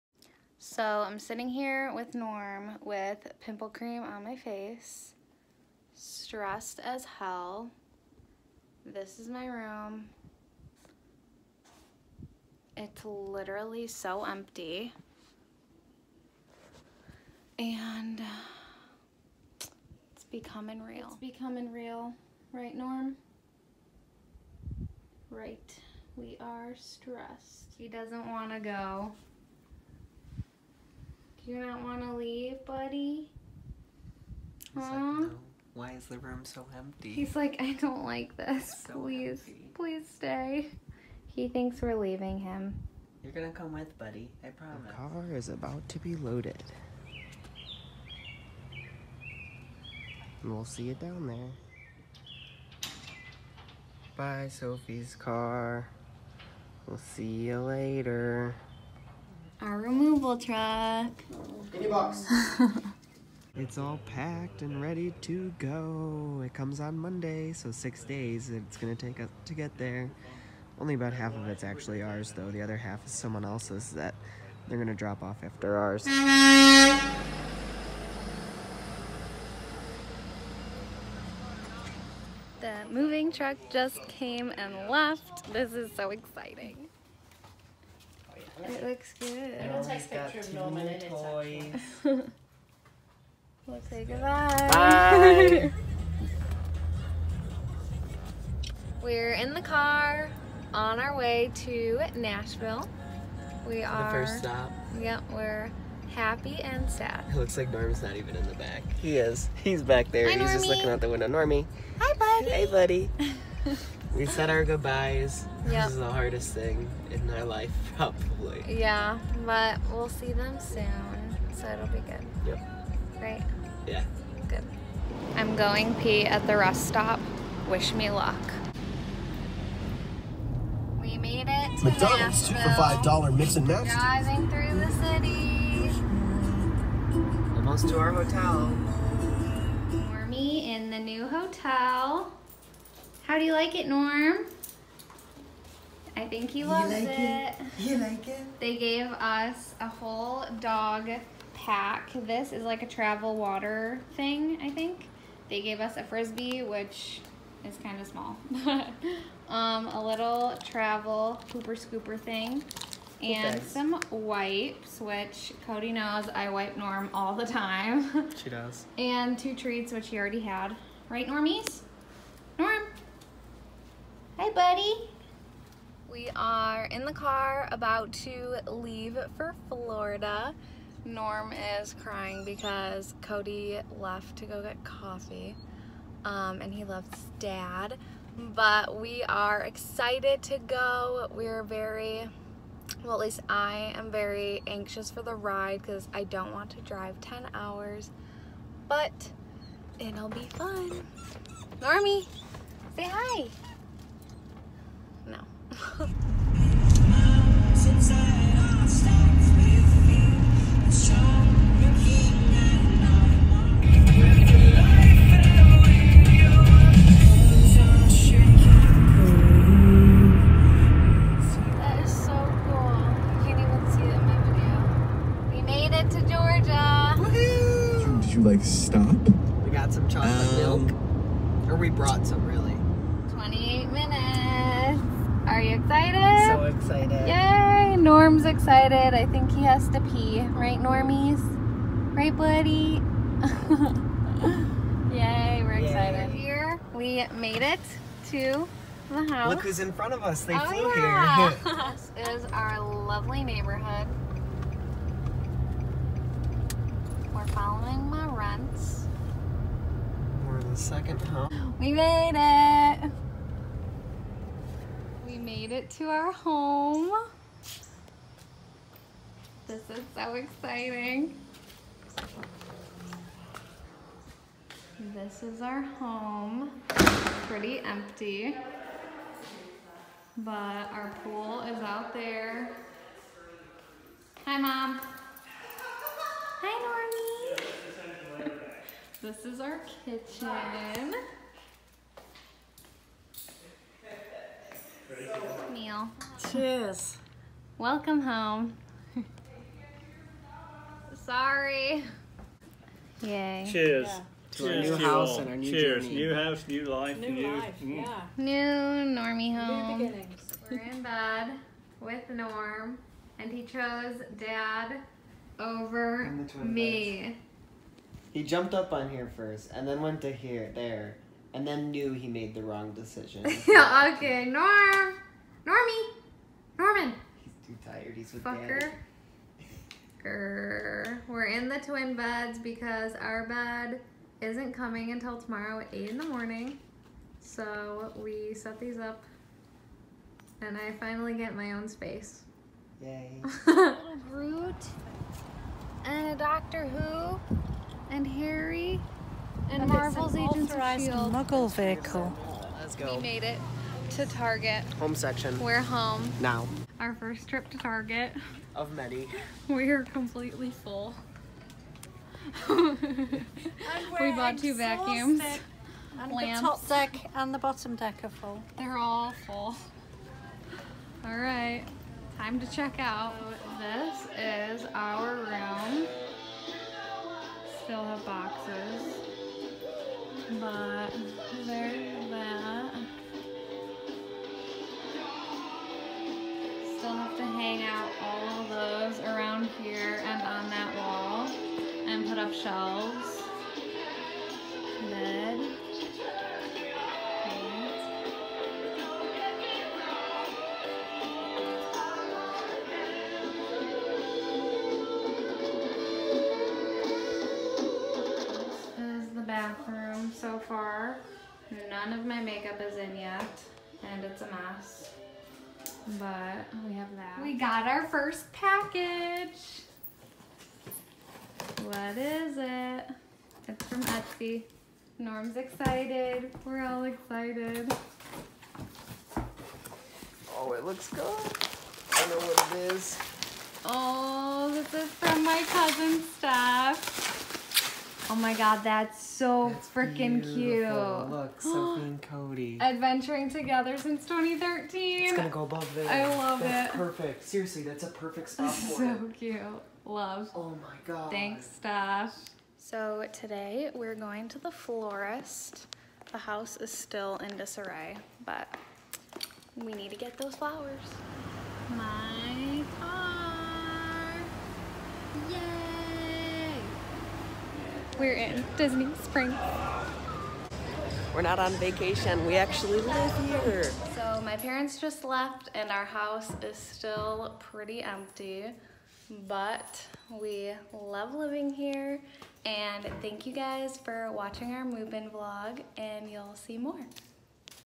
VLOG: We’re Moving to FL!
Hello!!

We did it- we MOVED TO FLORIDA! It was definitely a rough first move for us but we loved every second of it and are LOVING living here!